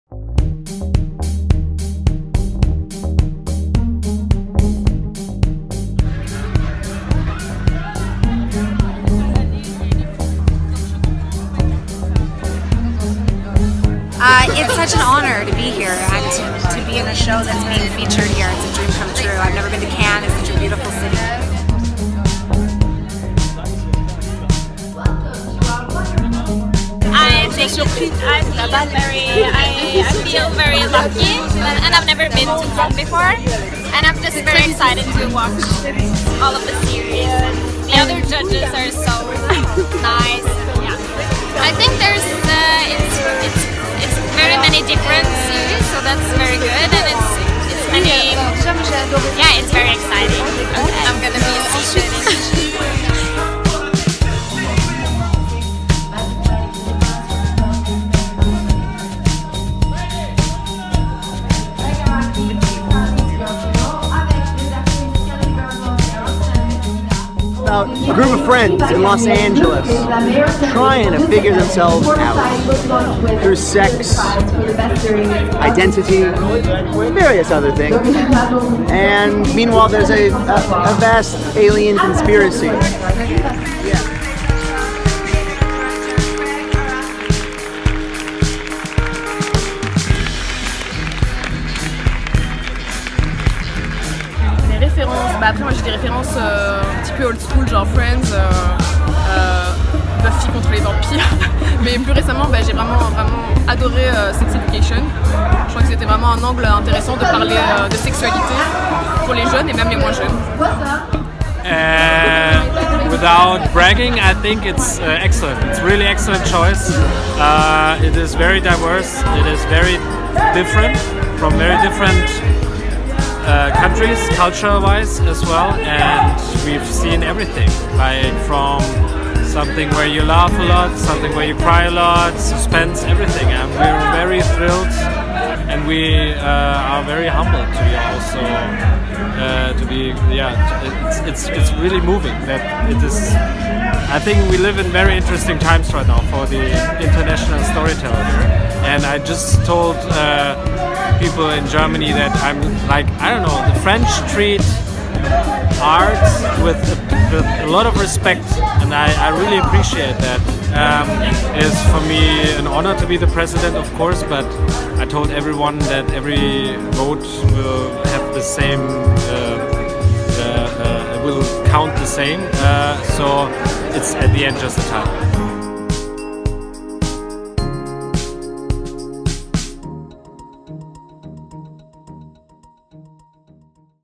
[BACK TO] CANNESERIES - Josefine Frida-Pettersen, Baran bo Odar, Natoo, Beau Mirchoff...
Retour sur le festival Canneséries qui s'est déroulé du 5 au 10 avril derniers. Les vidéos ont été réalisées par Just About TV sur le PINK CARPET de l'événement.

Interviews réalisées par : Agathe Hakoun

Just About TV est un site d'actualité et de critiques dédié aux séries entièrement géré par des bénévoles passionnés.